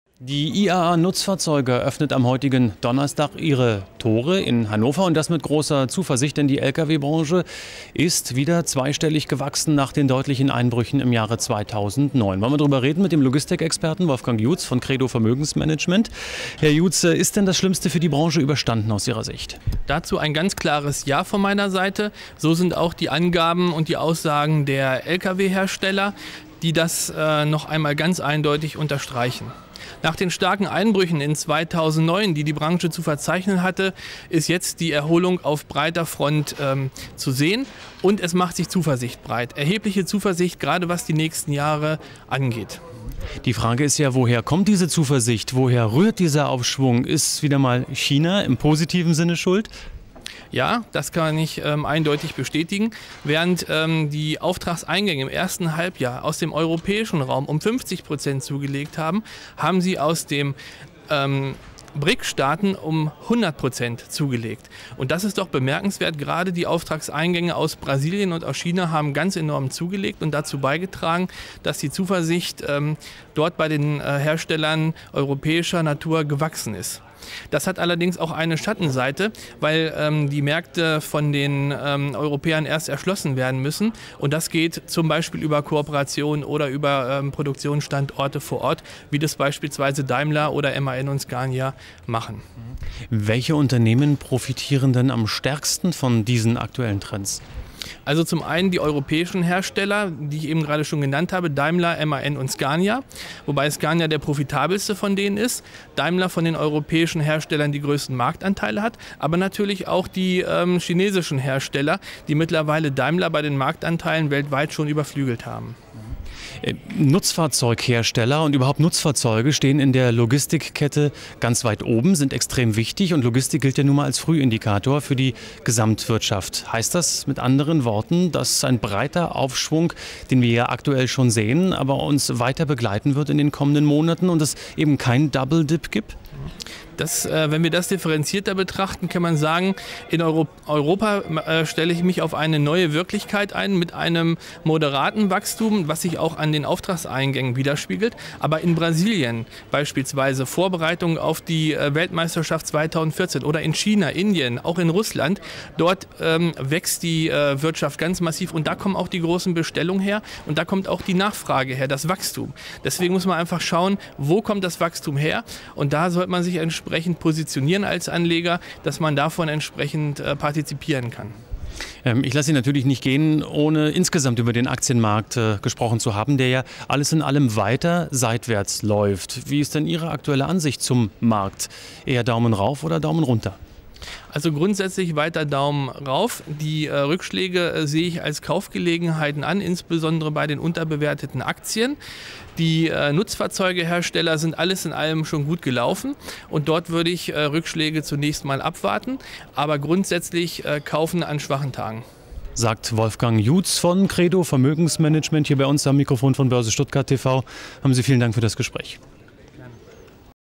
IAA Nutzfahrzeuge: Die Lust am Laster
Die IAA-Nutzfahrzeuge öffnet am heutigen Donnerstag ihre Tore in Hannover. Die Zuversicht in der Branche ist groß:
Schließlich sprechen zweistellige Zuwachsraten eine deutliche Sprache. Die Geschäftsführung von MAN hat bereits einen neuen Absatzrekord angekündigt. Rosige Zeiten für die Branchenriesen um Daimler, Scania und MAN? Eine Einschätzung von Wolfgang Juds, Credo Vermögenmangement, im Gespräch mit Andreas Franik.